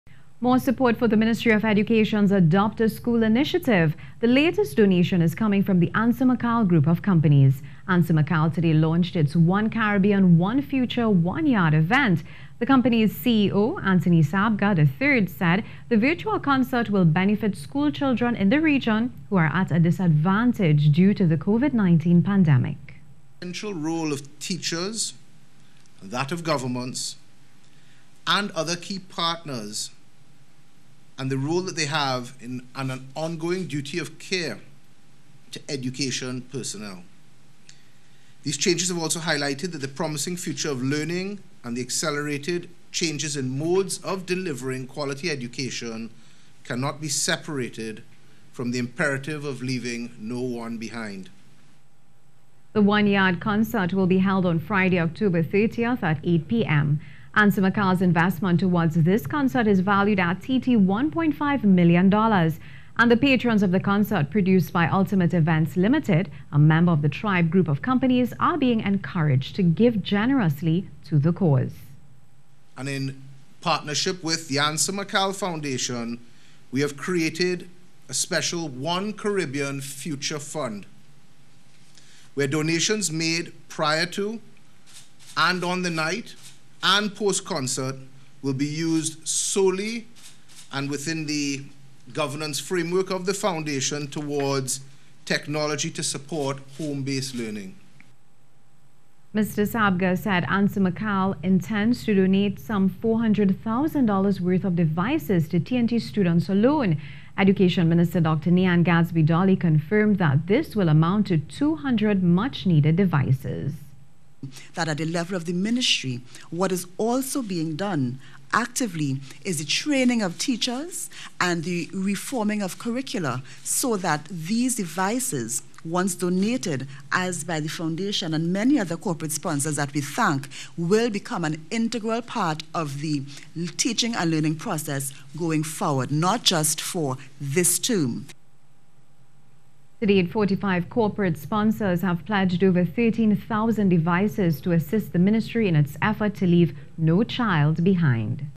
One Yard Virtual Concert To Benefit Children In Need Of Laptops
More support for the Ministry of Education's Adopt-A-School initiative with the latest donation coming from the ANSA McAL Group of Companies.

ANSA McAL today launched its "One Caribbean, One Future, One Yard" event. The company's CEO, Anthony Sabga III, said the virtual concert will benefit school children in the region who are at a disadvantage due to the COVID-19 pandemic.

The concert will be held on Friday October 30th 2020 at 8pm.

ANSA McAL's investment towards this concert is valued at TT$1.5 million.

Patrons of the concert produced by Ultimate Events Ltd, a member of the Tribe Group of Companies, are being encouraged to give generously to the cause.

Mr. Sabga says  ANSA McAL intends to donate some $400,000 worth of devices to T&T students alone.

Education Minister Dr. Nyan Gadsby Dolly confirms that this will amount to 200 much-needed devices

To date, 45 corporate sponsors have pledged over 13,000 devices to assist the Ministry in its effort to leave no child behind.